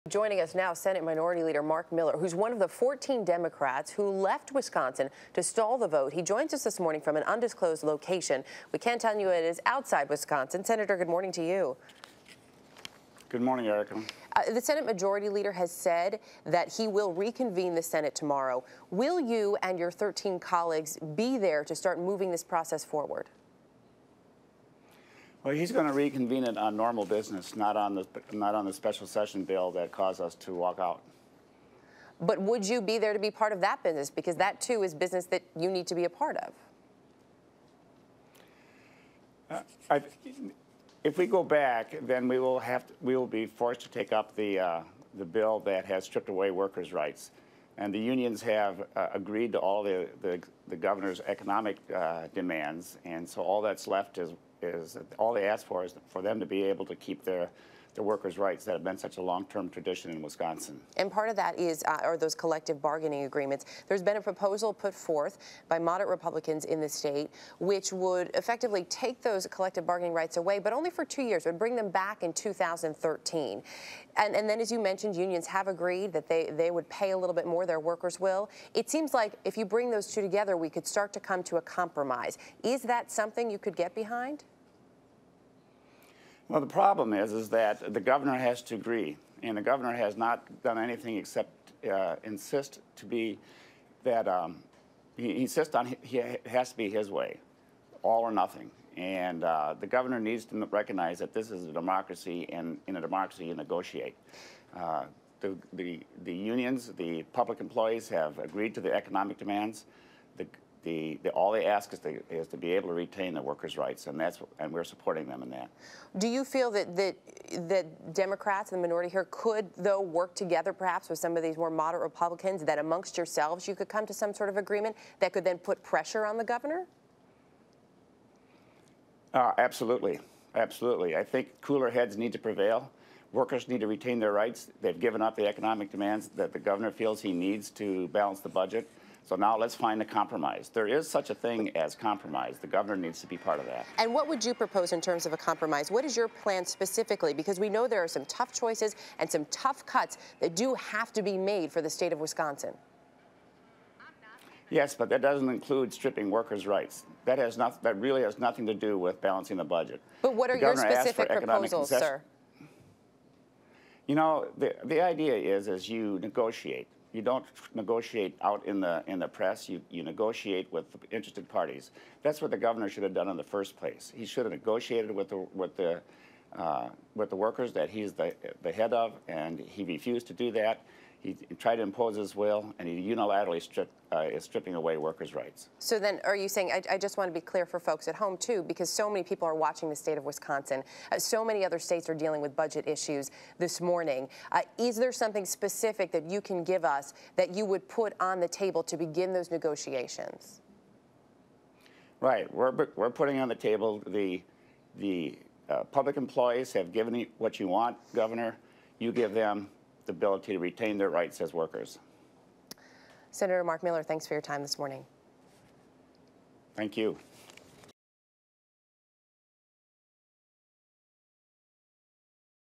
Wis. Sen. Mark Miller: "In a democracy, we negotiate"
Erica Hill talks to Mark Miller, the Wisconsin State Senate Minority Leader who left the state with thirteen other Democrats to stall a budget vote that they say strips away rights from union workers.